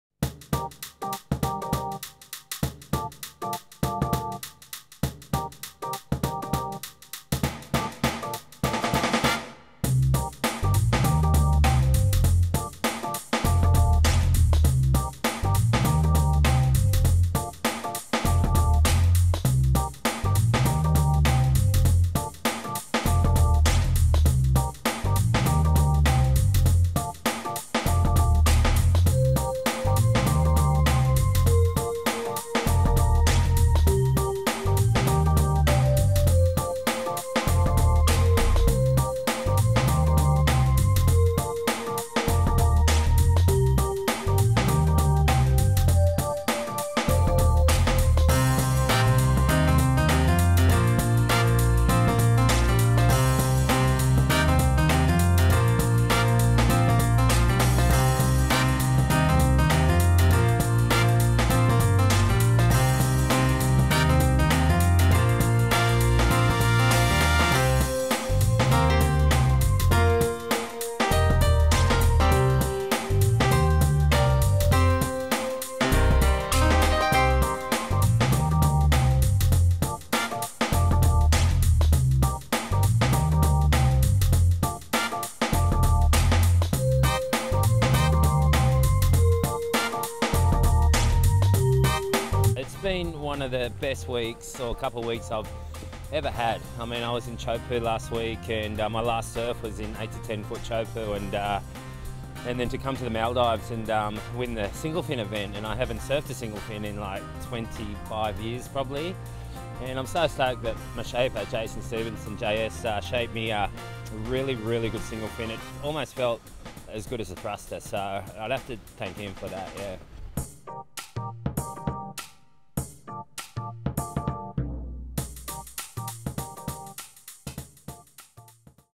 Single Fin - Final - Four Seasons Maldives Surfing Champions Trophy
Surfing Champions Trophy Single Fin Division, Occy wins at Sultans on day 1 of competition.